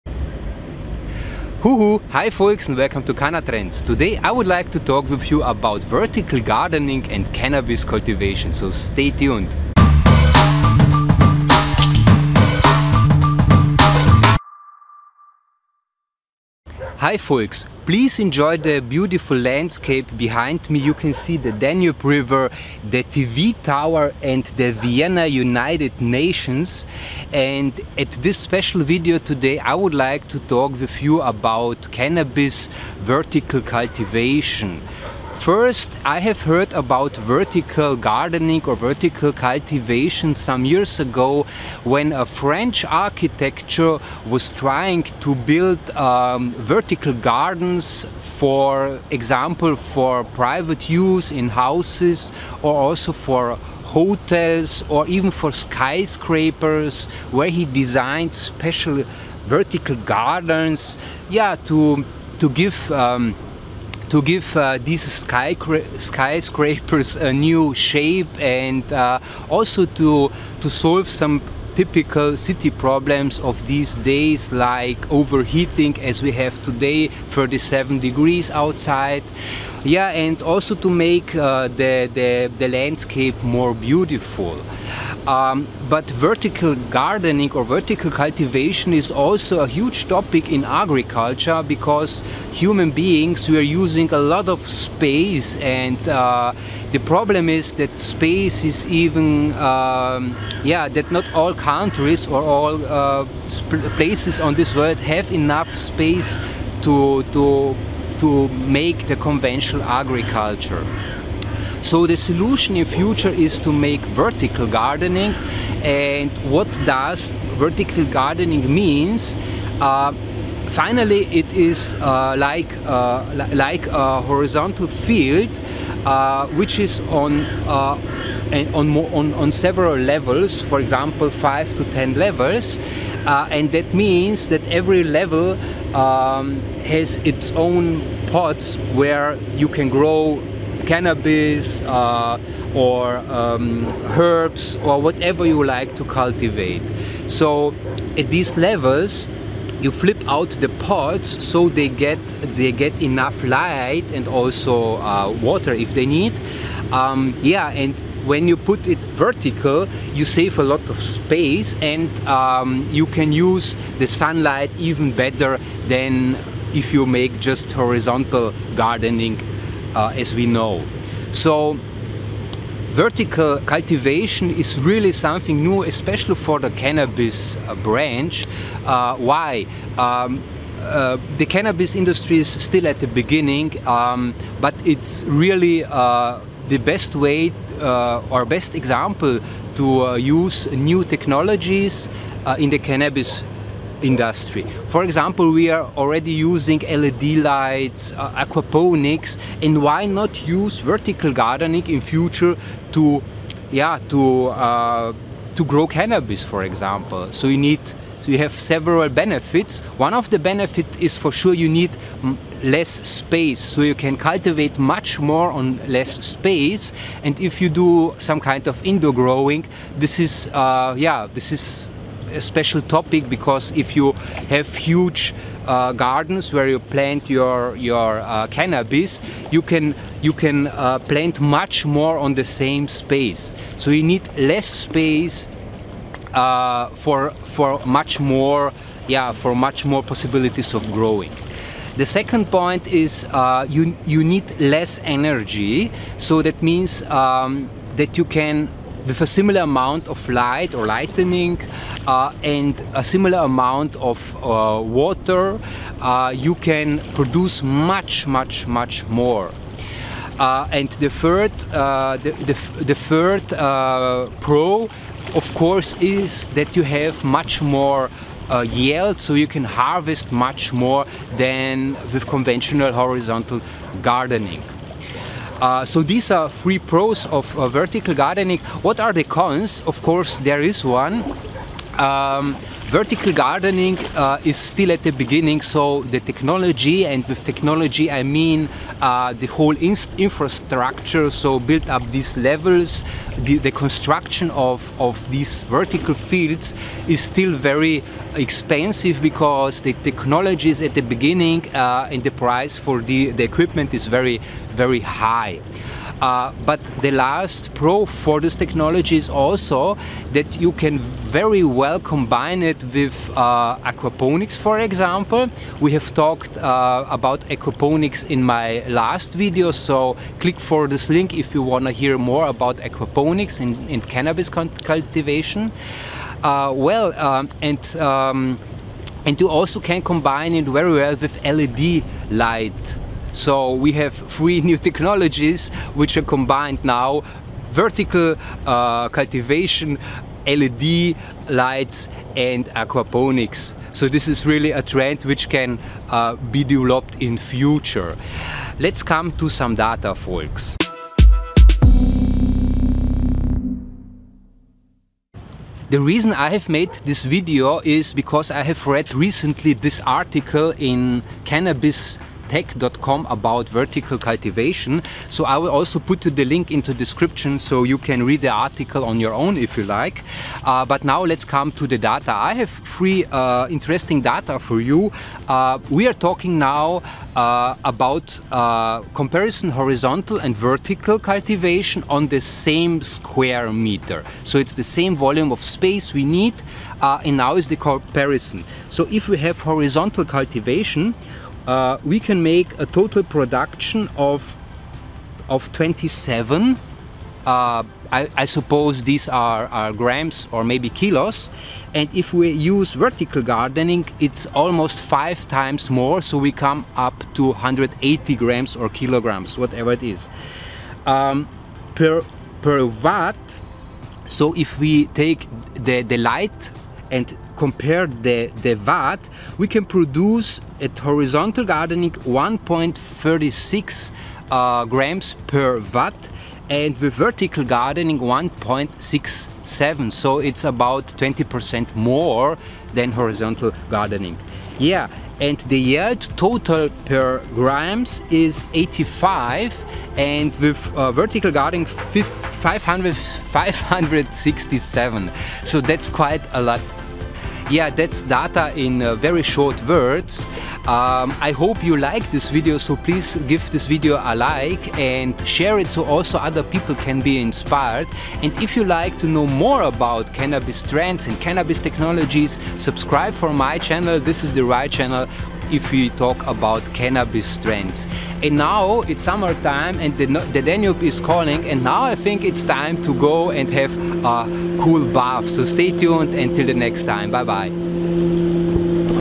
Vertical Cannabis Cultivation
High Folks,

LED, aquaponics and now vertical gardening - these are the total new trends in cannabis cultivation and will change the industry within the next years. In this video I´ll talk about vertical cannabis cultivation and it´s pros and cons.